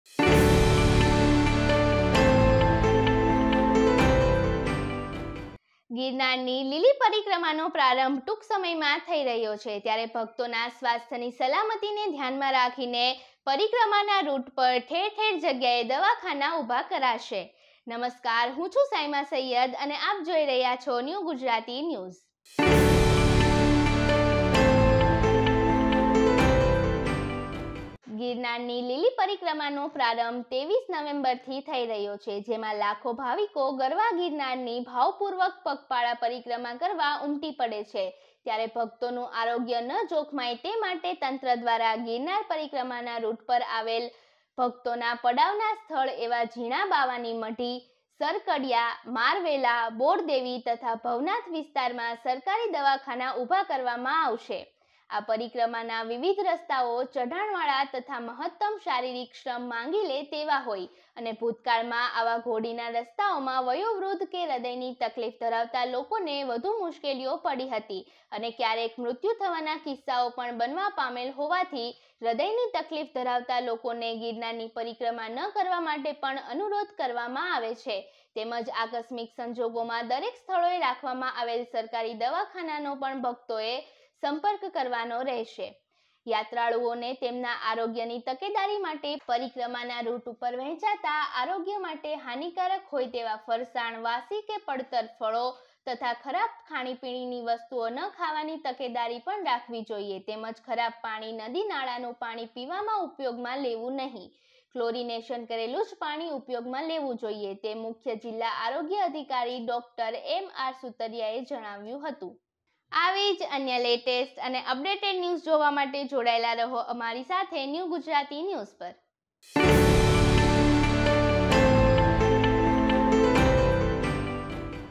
ગિરનારની પરિક્રમમા રૂટ ઉપર ભક્તોના સ્વાસ્થ્યની સલામતી માટે દવાખાના ઉભા કરવામાં આવશે !
ગિરનાર પરિક્રમા રૂટ પર જીણાબાવાની મઢી, સરકડીયા, મારવેલા, બોરદેવી અને ભવનાથ વિસ્તારમાં યાત્રાળુઓ માટે આરોગ્ય સુવિધાઓ ઉભી કરવામાં આવશે. તેમજ પરિક્રમાનો માર્ગ કઠિન હોય વયોવૃદ્ધ અને હૃદયરોગની બીમારી ધરાવતા ભાવિકોને પરિક્રમા ન કરવા સલાહ આપવામાં આવી છે.

New Gujarati News
NGN
today news
gujarati news
trending news
viral video
viral news
top news
best news
breaking news